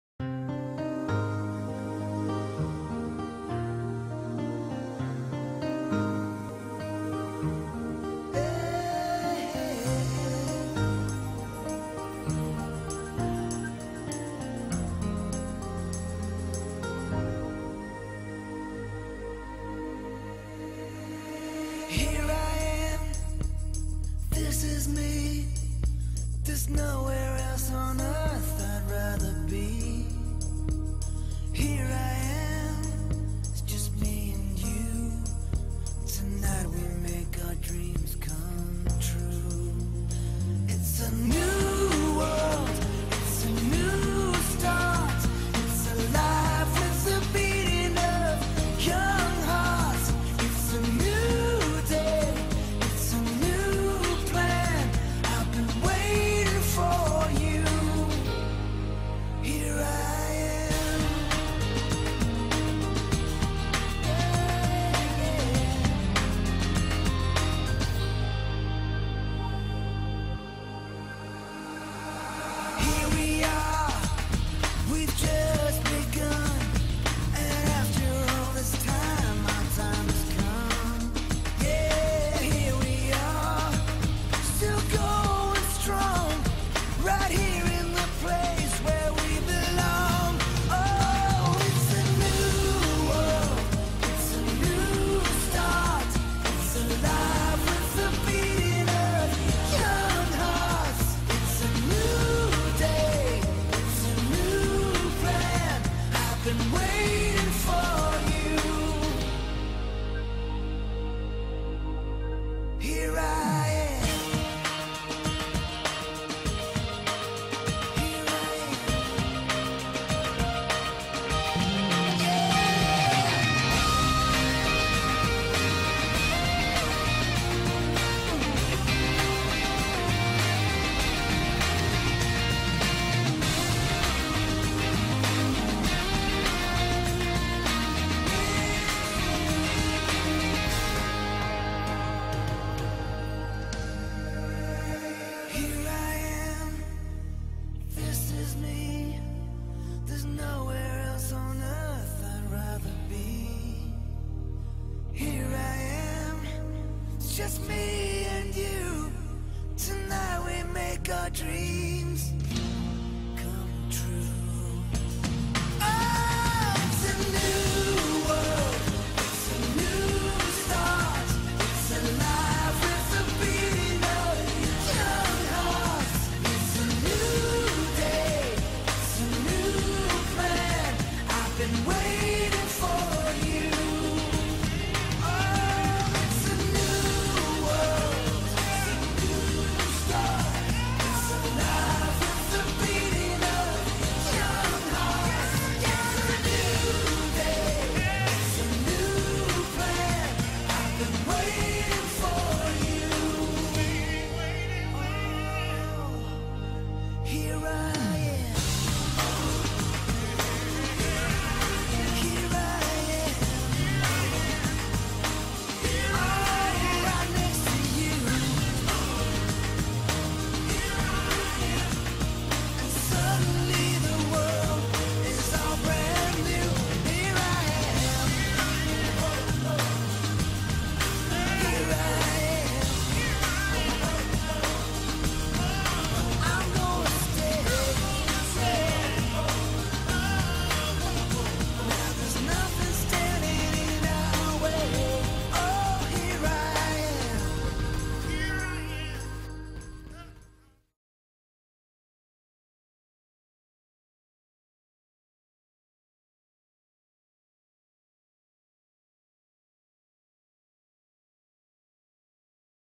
Whisper The Filly - Music Video Of A Horse
This is my Filly Whisper, she is 2 Years old unbroken Blue Roan Filly, Whisper has a twin brother called Orica. I got a video camera for Christmas so on Christmas Day I  took my Video Camera out and took LOADS of video's of her :DD